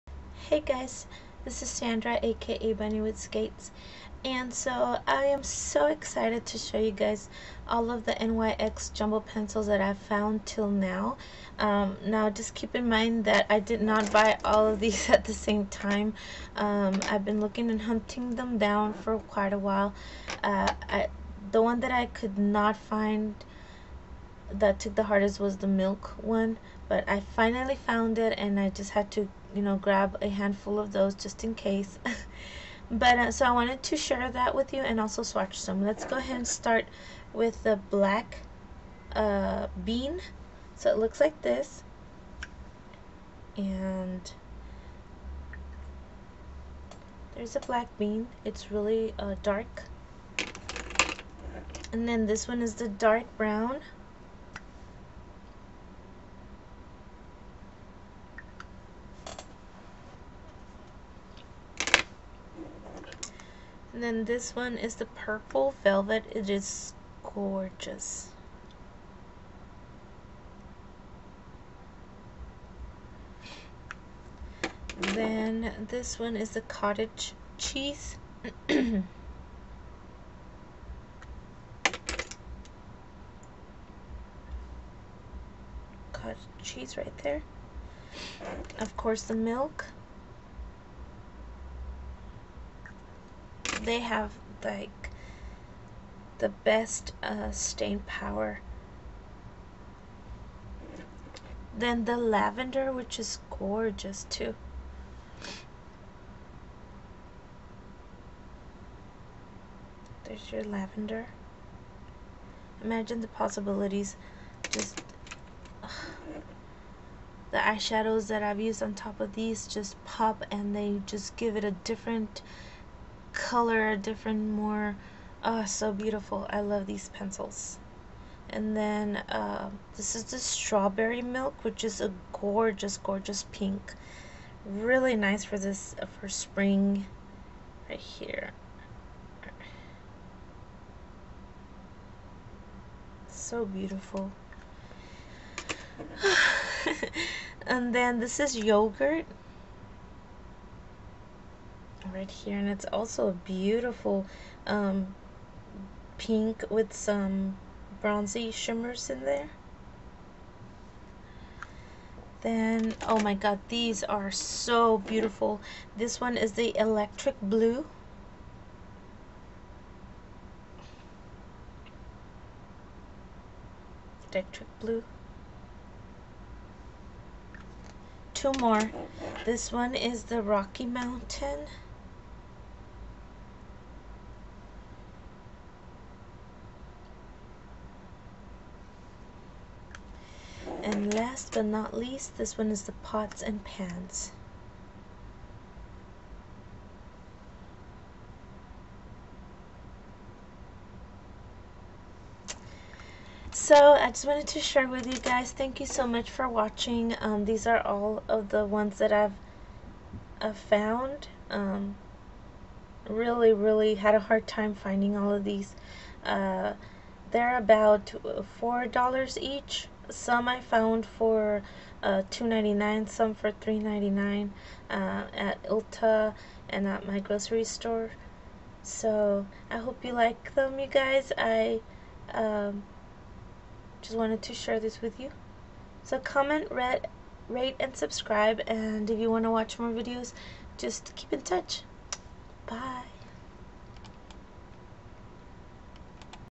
NYX Jumbo Pencil Swatches,...yay!☺♥
I had a hard time finding these, hope you enjoy☺♥
Black bean
Dark brown
Purple velvet
Cottege cheese
Milk
Lavender
Strawberry milk
Yogurt
Electric blue
Rocky mountain